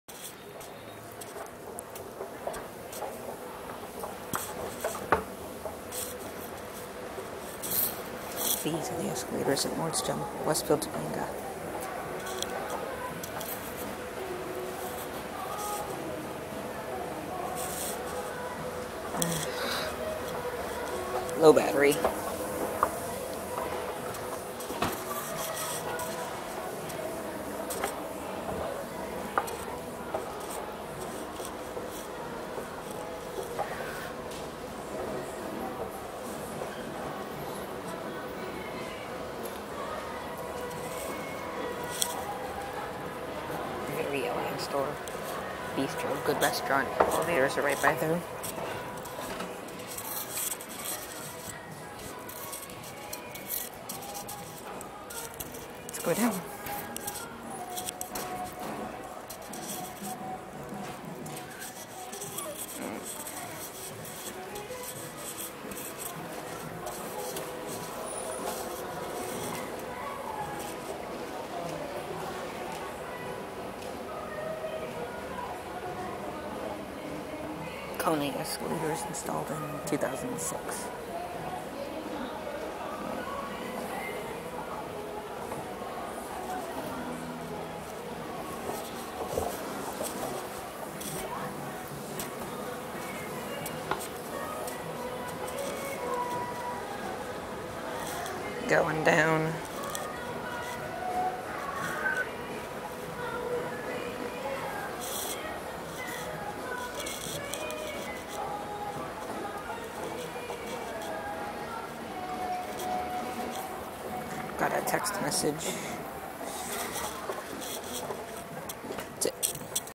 Kone Escalators - Nordstrom - Westfield Topanga - Canoga Park, California
Happy Escalator Monday!!

Filmed on iPod Touch on May 22, 2011.